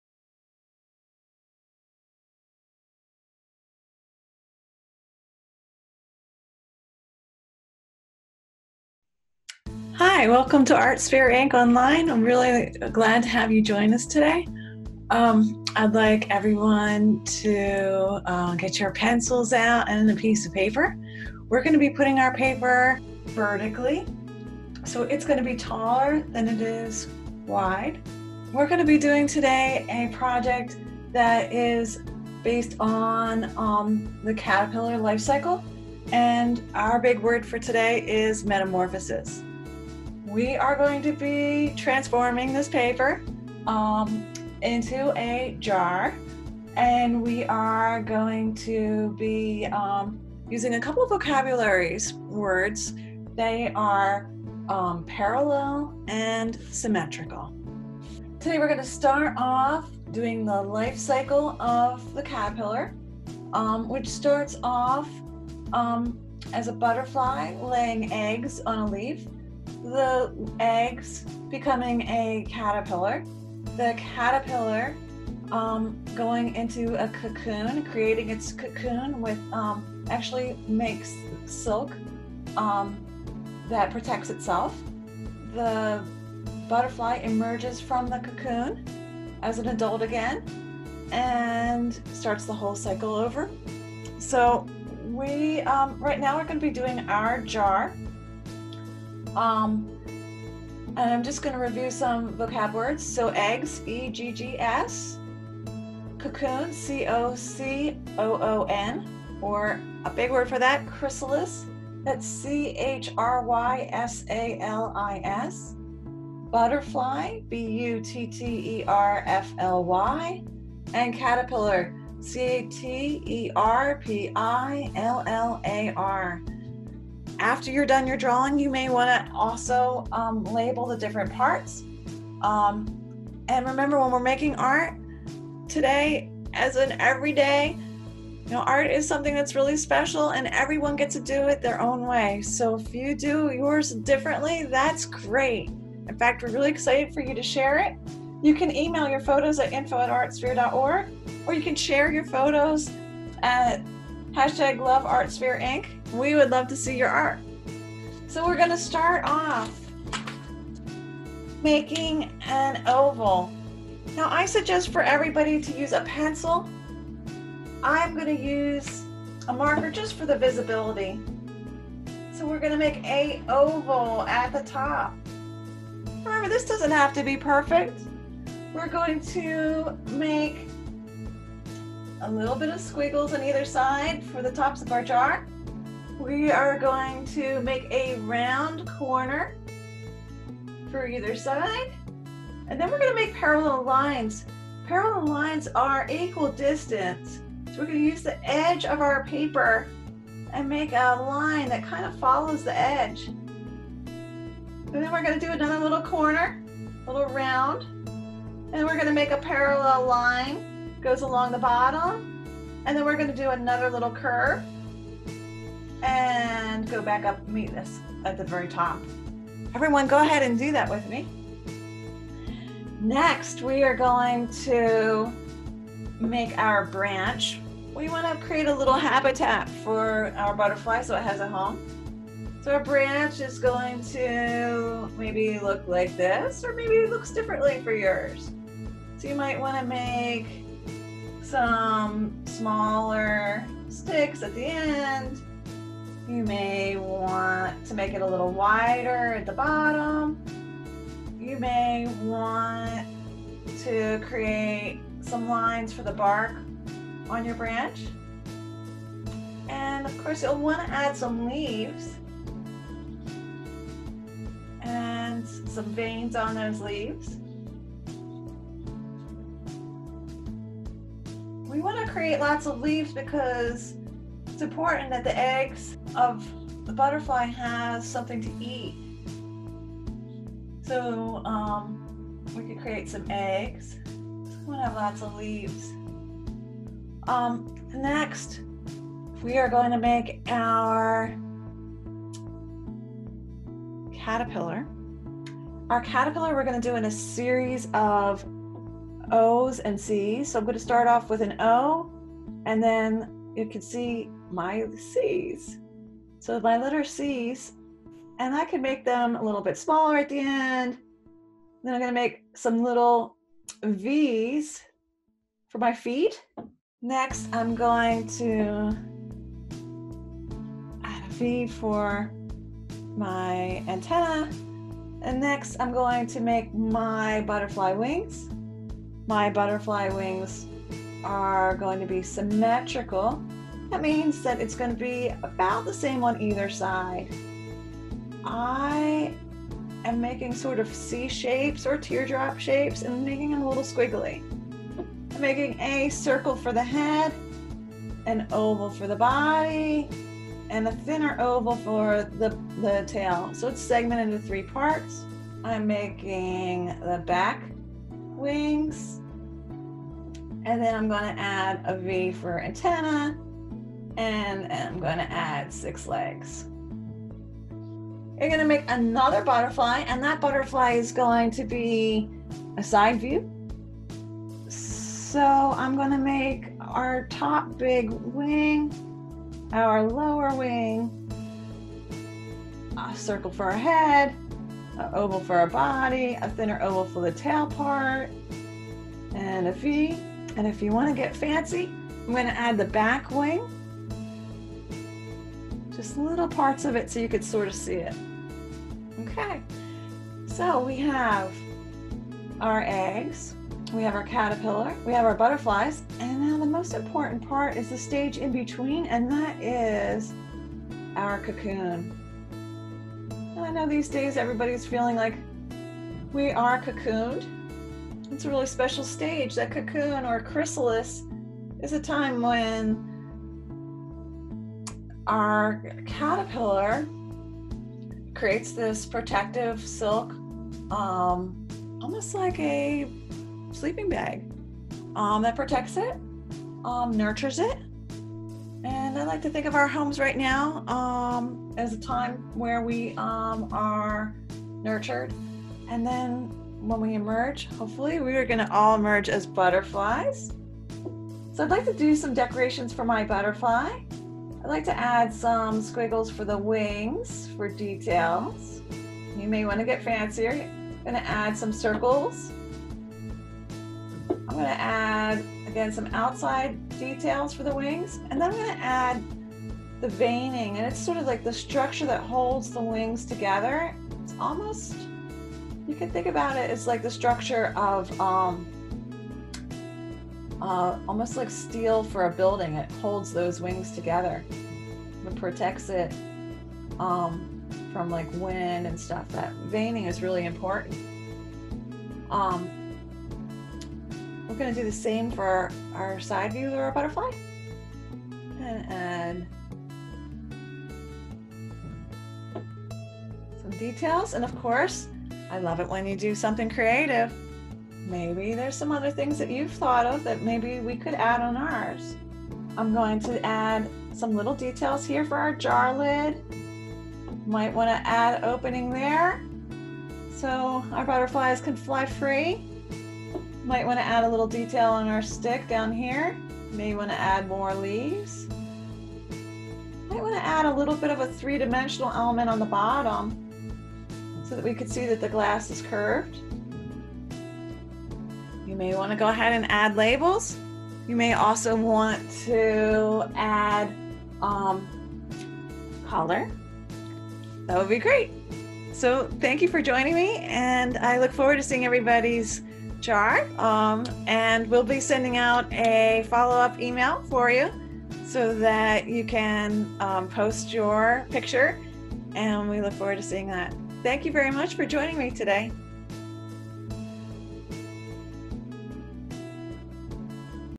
Draw a Butterfly jar with ASI!
Join ASI Teacher, Kristin Groenveld, and learn how to draw a butterfly jar. There will be a second video to drawing the butterfly life cycle in a jar.

Vocab Words:
Parallel- Lines that never intersect and are also equally apart from each other.
Symmetrical- Made up of exactly similar parts facing each other or around an axis; showing symmetry.
Eggs- The female butterfly attaches the eggs to leaves or stems of plants that will also serve as a suitable food source for the larvae when they hatch.
Cocoon- A silky case spun by the larvae for protection in the pupal stage.
Chrysalis- The transitional state where the butterfly is transforming.
Butterfly- A nectar-feeding insect with two pairs of large, typically brightly colored wings that are covered with microscopic scales.
Caterpillar- The larva of a butterfly.

Share your finished artwork or selfie with us using